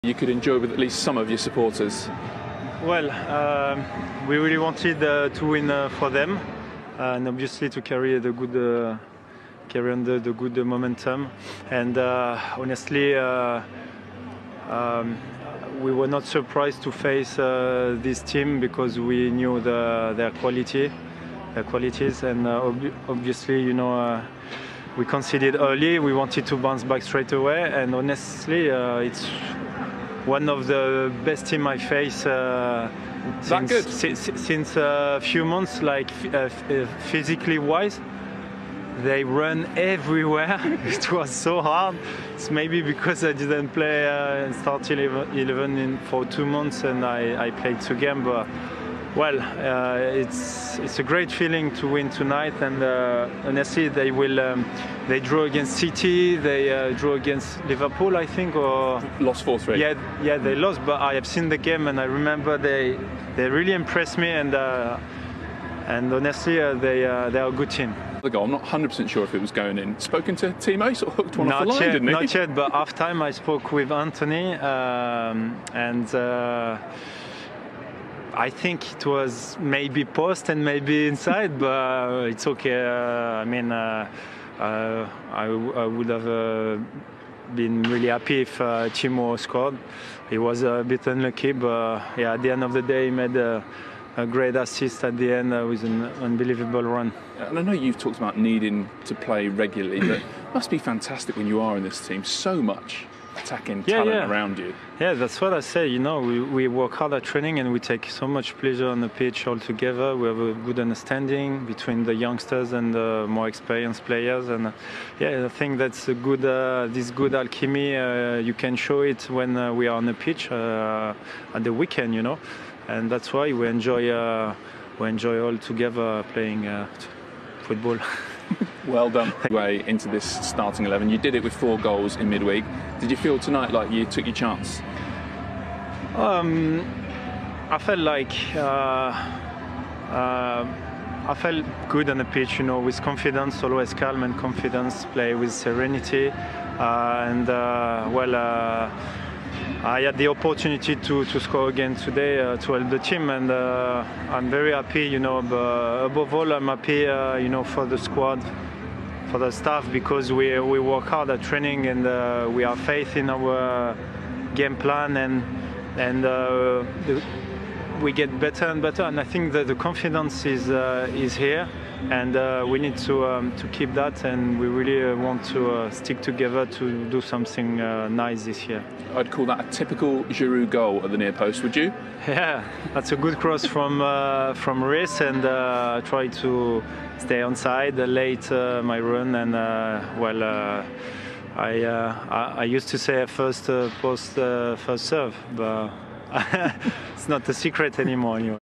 MAN OF THE MOMENT OLIVIER GIROUD - LEEDS ARE THE MOST DIFFICULT TEAM IN THE PL
OLIVIER GIROUD INTERVIEW
CHELSEA 3-1 LEEDS